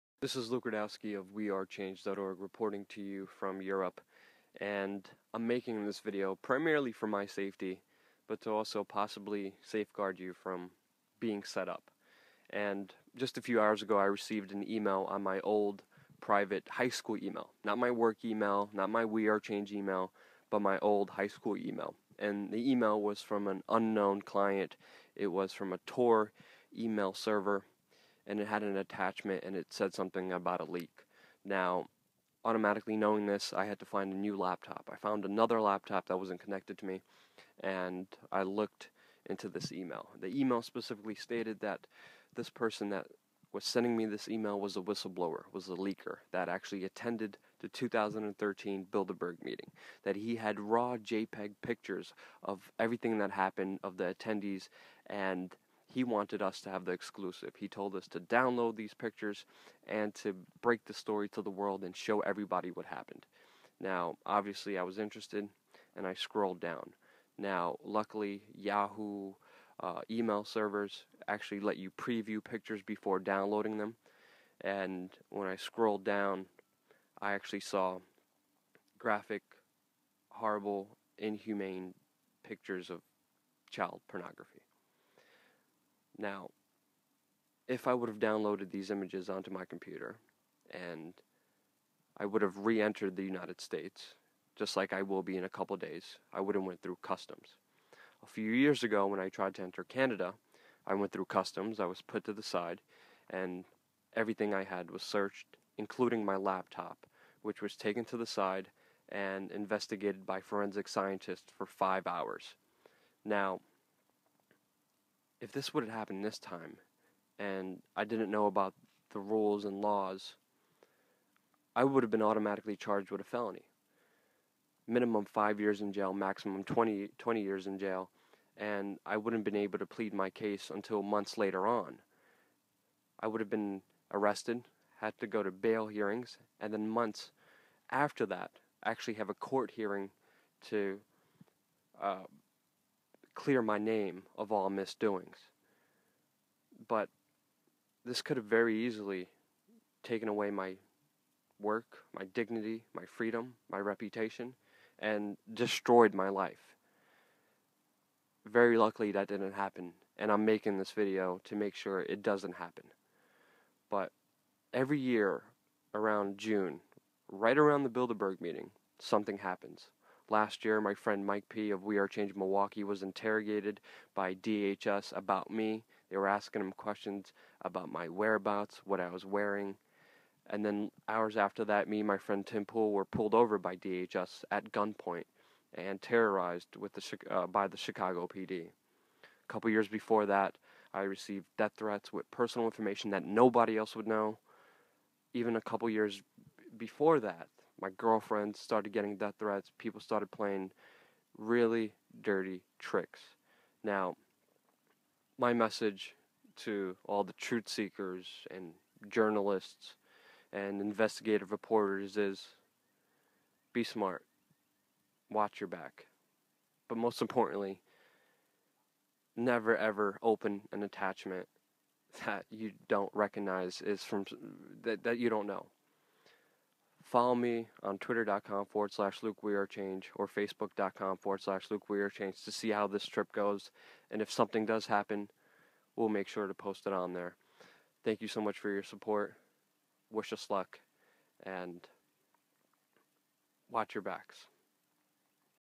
Attempted setup of Luke Rudkowski of We Are Change
A few hours before making this video, Luke Rudkowski of WeAreChange.org received an email from an anonymous sender claiming to be a Bilderberg whistleblower with attached photos from inside the 2013 Bilderberg meeting at the Grove Hotel in Watford, England. Luke opened the email from another computer and itl stated that he was a whistleblower and wanted Luke to break the story so he was sending him these photos exclusively and specifically stated that Luke needed to download the photos. Fortunately before downloading, Yahoo gives a preview of the photos and the photos were actually graphic child pornography. Luke has had his computer searched by forensics when going into Canada to cover the G20 when they searched him and interrogated him for hours, something like this could happen again when we come back to the U.S. from travelling Europe. Had this been the case, even with deleting, they would still find the images. There would be no understanding of the true story and he would be facing a felony charge. Besides that also completely jeopardizing his work, reputation and livelihood. We wanted to make this public for Luke's safety and also for people to be aware that this kind of thing happens. We do not know who this was, all we know is that it is so important for anyone in this line of work to remain extremely vigilant.

for any updates to this